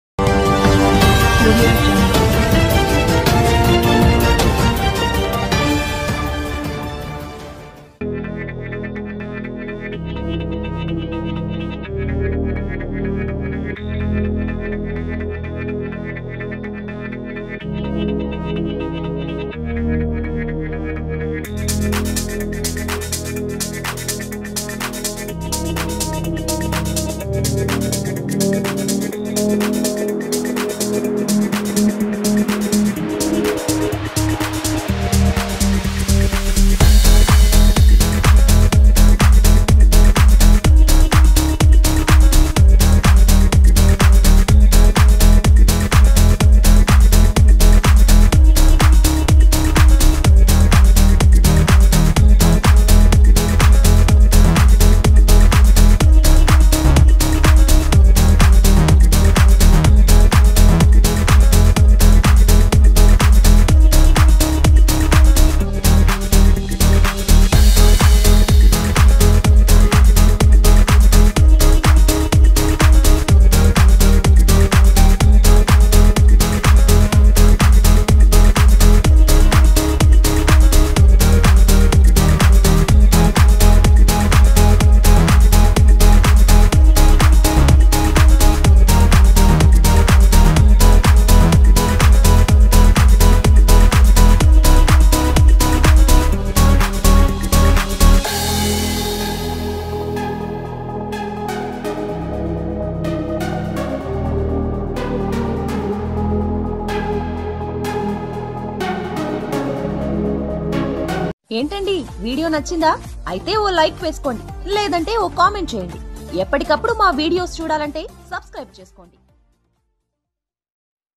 చిరంజీవి పాటకు స్టెప్పులేసిన నాగబాబు నిహారిక | Niharika Dance With Chiranjeevi At Mehandi Function
Watch#